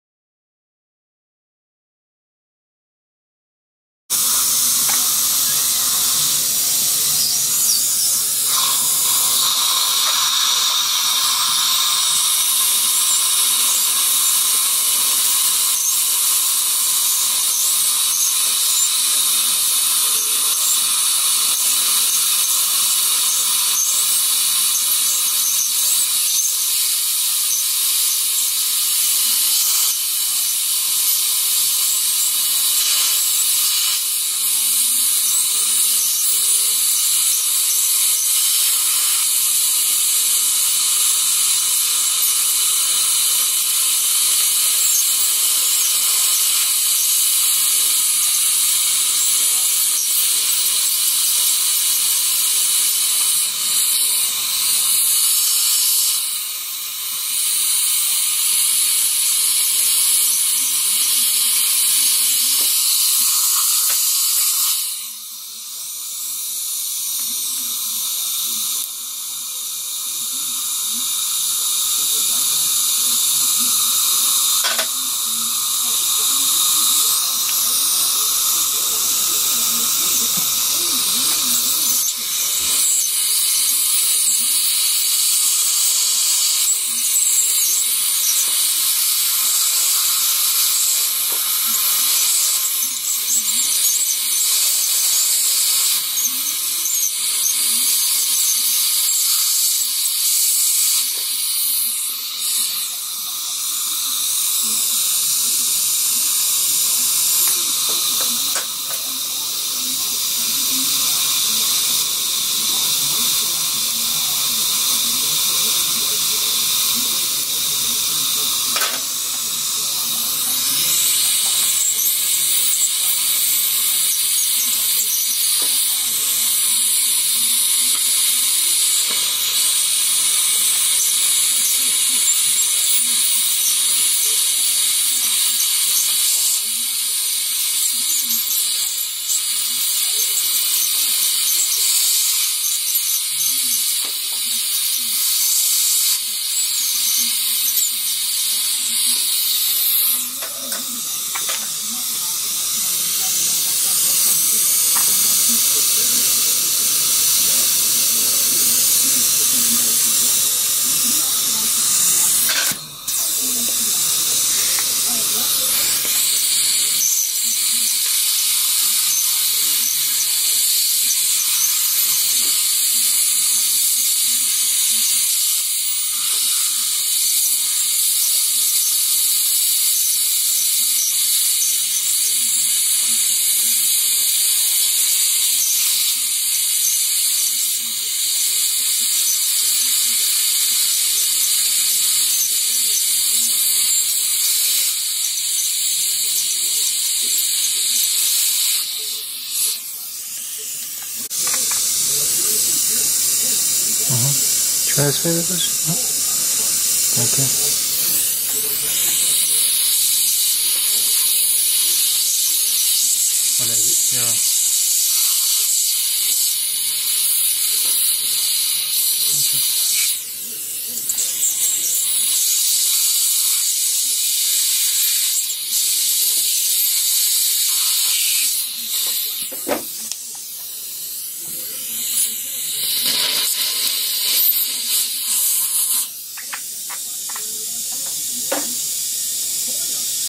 Removing Post Using NSK Piezo Unit
I used NSK Piezo Unit to remove post